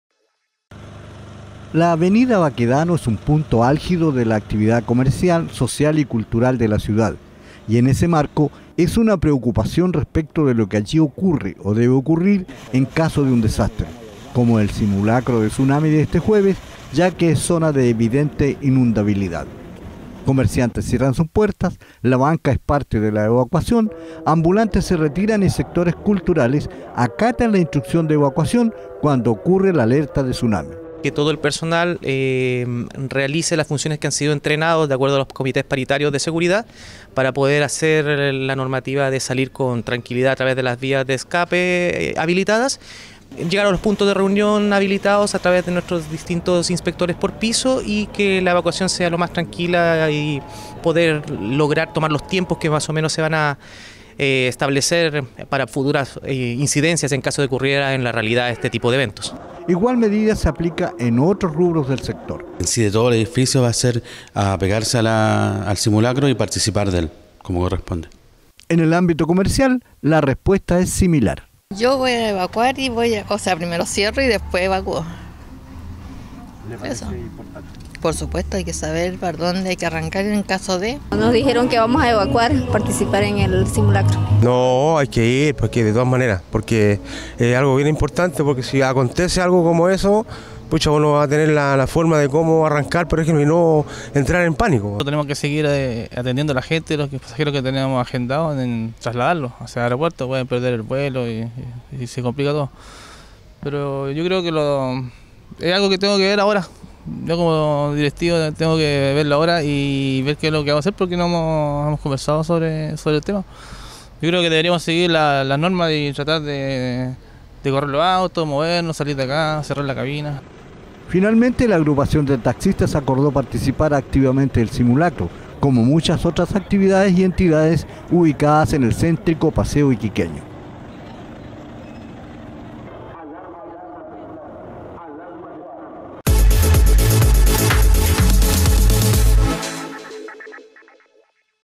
EMPRESAS Y COMERCIANTES FUERON PARTE DEL SIMULACRO (2017.06.15) Lengua de señas
La avenida Baquedano es un punto álgido de la actividad comercial, social y cultural de la ciudad, y en ese marco, es una preocupación respecto de lo que allí ocurre o debe ocurrir en caso de un desastre, como el simulacro de tsunami de este jueves, ya que es zona de evidente inundabilidad.

Comerciantes cierran sus puertas, la banca es parte de la evacuación, ambulantes se retiran y sectores culturales, acatan la instrucción de evacuación, cuando ocurre la alerta de tsunami.